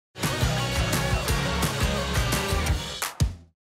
Lawlors Hotel Naas How to Make a Cocktail series
This is a short clip from our  How to series, Dan is making a classic cocktail the Old Fashioned in the Random Inn